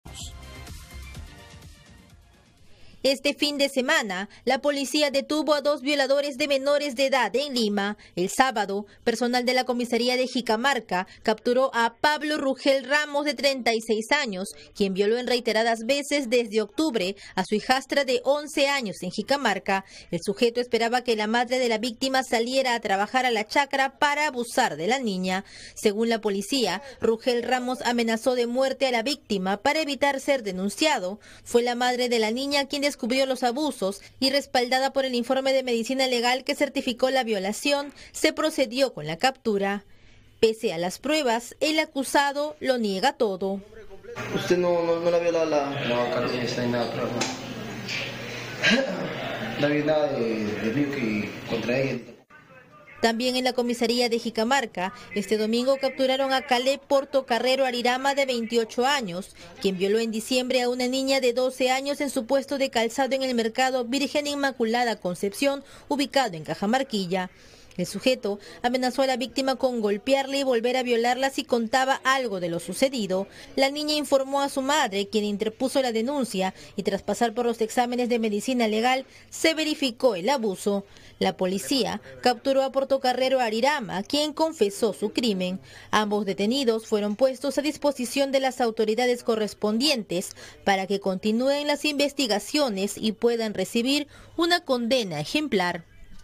Jicamarca: Policía capturó a dos sujetos acusados de violar a menores de edad
Uno de los agresores negó los hechos, pese a que los exámenes médicos comprobaban el delito. El segundo admitió que sí abusó sexualmente de la menor.